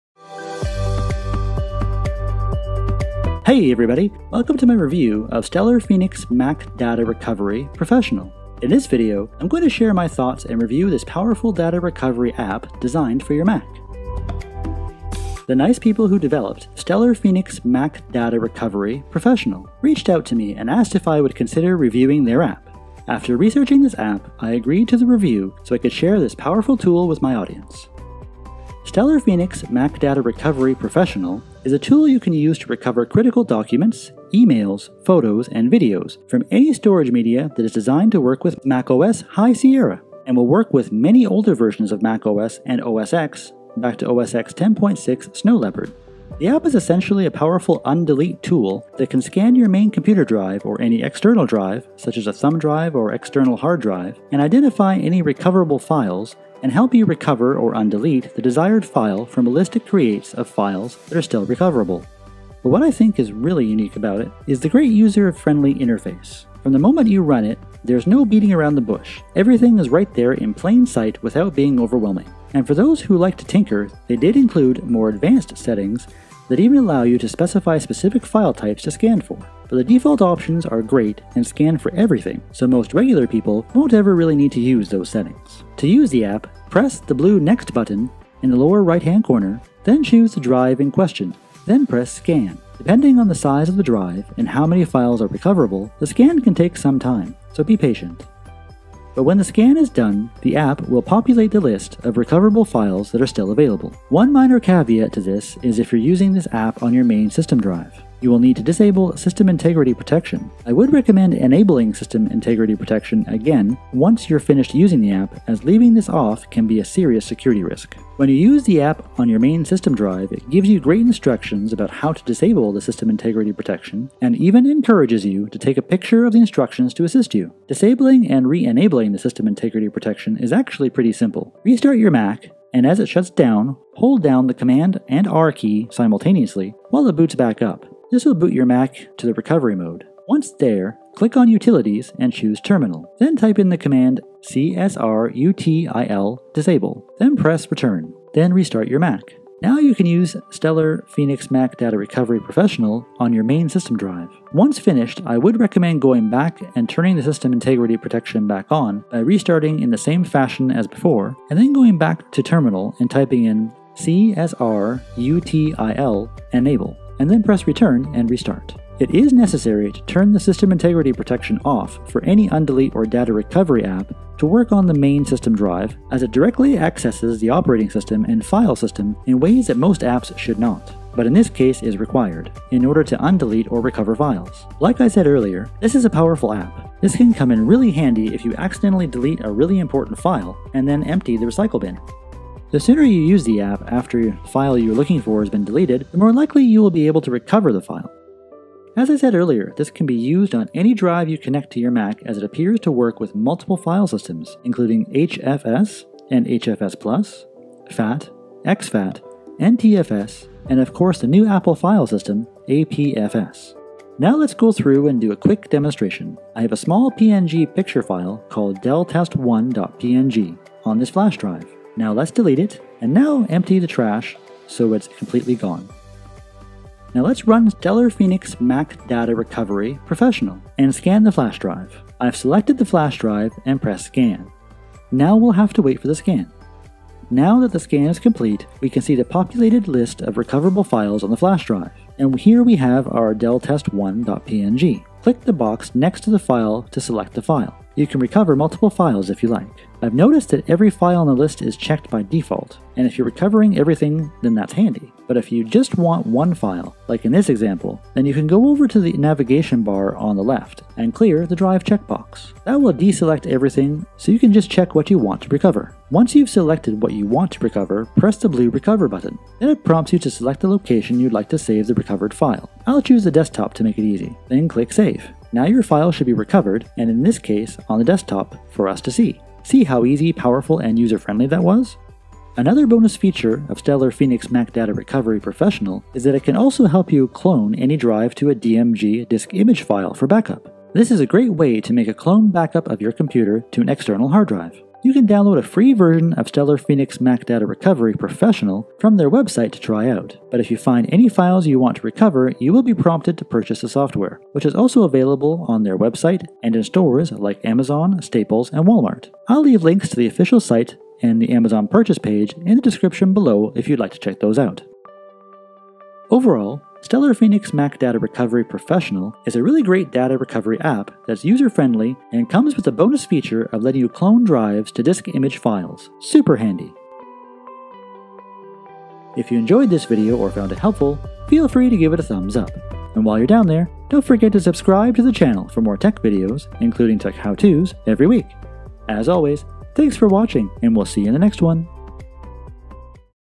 Best MacBook Data Recovery Software | Recover MacBook 2018, 2017 and so on
Bullet Points from the video:

1. This tool recovers critical documents, emails, photos and videos

2. The Software is designed to work with OS High Sierra

3. Further, it works with older version - OSX 10.6,10.7, 10.8, 10.9, 10.10, 10.11, 10.12

4. This app is powerful undelete tool that scans your main computer drive or external drive such as thumb drive to recover deleted files

5. It has unique and excellent user-friendly interface (no beating around the bush) everything is right in plain sight without being overwhelming

6. Includes more advanced settings that can add/edit new file types

7. Easy to run the scan or deep scan on any drive or media

8. For macOS High Sierra, it’s mandatory to disable SIP temporarily. Once data recovery is done enable SIP.

9. This software can be used on any drives including multiple file systems such as HFS, HFS+, FAT, ex-FAT, NTFS and of course the new Apple file system APFS.

Stellar Phoenix Mac Data Recovery Professional is Super-Handy.